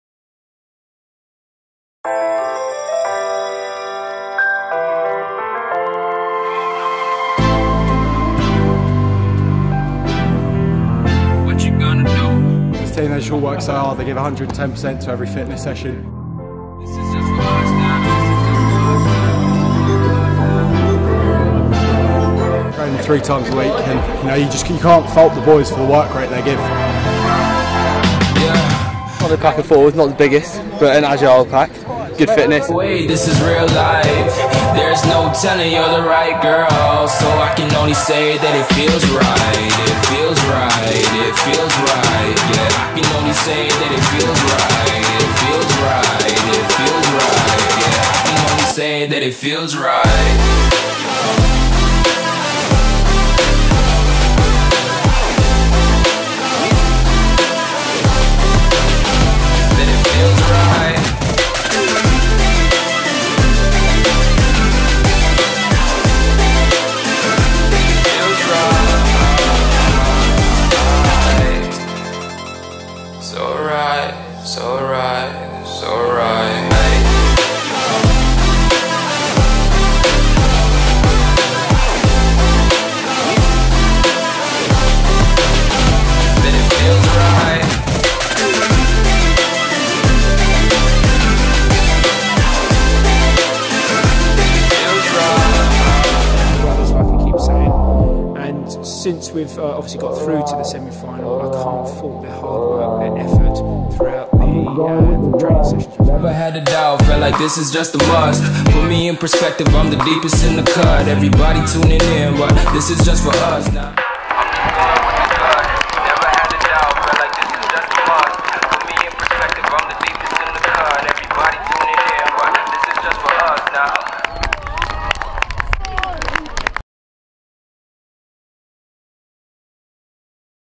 Felsted 1st XV: Road to Allianz
This video shows the Felsted 1st XV gearing up for their massive Natwest Cup semi final match vs Warwick School. With thanks to Will Alexander and Harry Beaton-Brown.